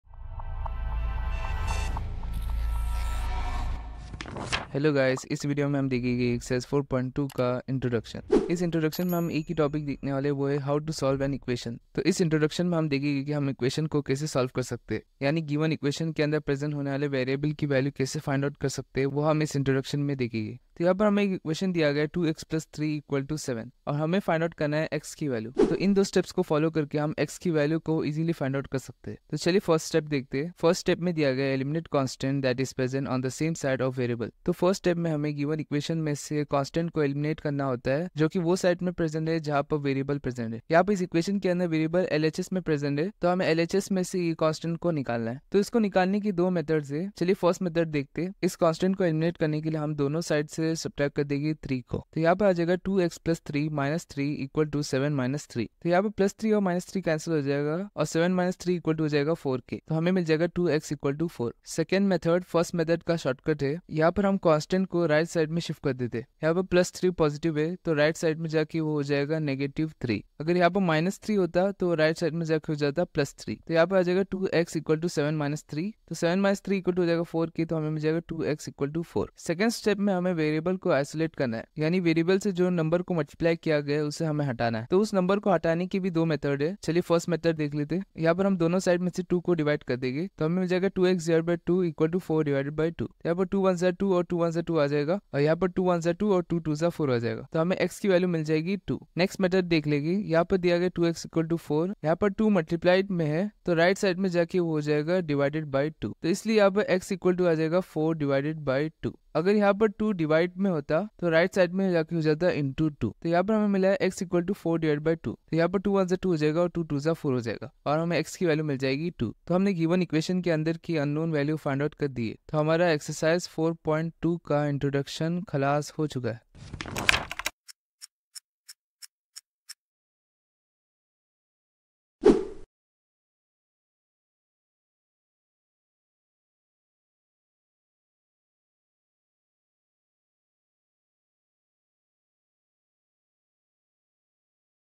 Introduction | Ex 4.2 | Chapter 4 - Simple Equations | Class 7th Maths | NCERT/CBSE | 2024
Welcome to our Class 7 Maths series on Chapter 4: Simple Equations. In this video, we kick off with an introduction to Exercise 4.2.

Join the Telegram Channels Now!

Time Stamps:
0:00 Intro
0:08 How to solve an equation?
0:25 Step 1
1:19 Step 2
2:06 Outro